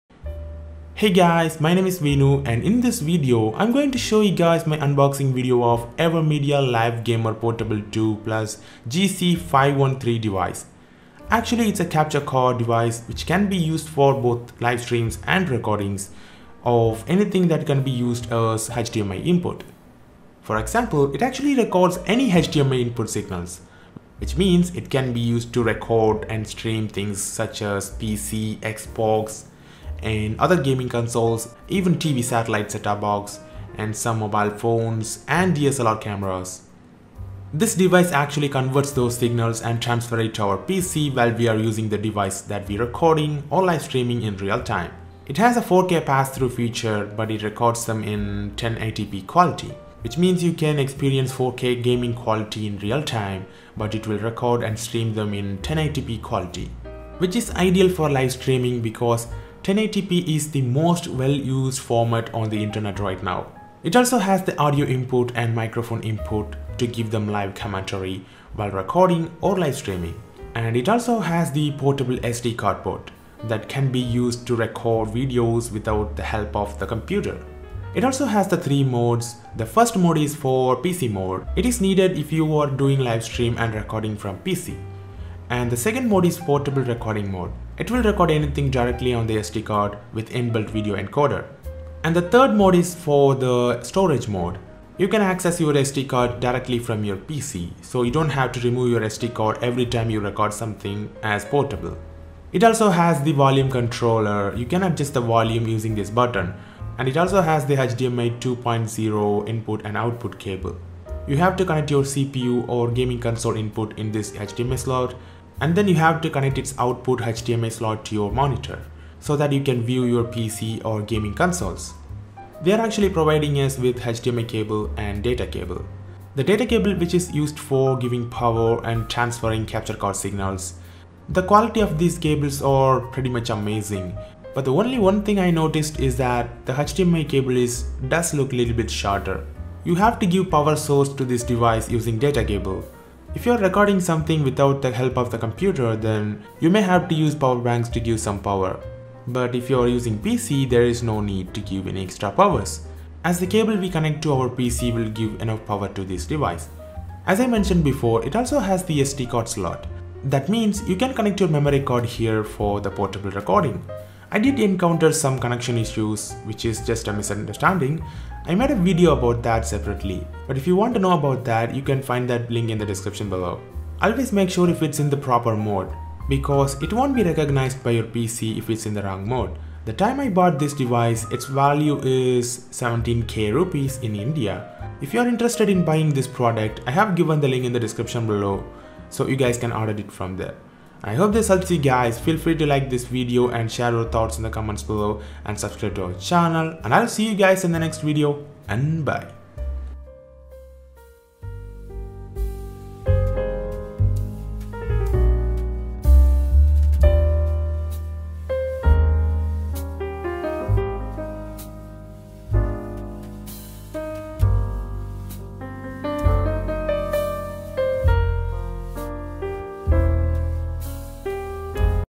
AVerMedia Live Gamer Portable 2 Plus, 4K Pass-Through Capture Card Unboxing & Review
AVerMedia Live Gamer Portable 2 Plus Model - GC513, 4K Pass-Through, Full HD 1080p60 USB Game Capture, Ultra-Low Latency, Record, Stream, Plug & Play Unboxing, and Review. I have explained all the features and specifications of this capture card. Watch this entire video to learn more about it.

To Get your own AVerMedia Capture Card Device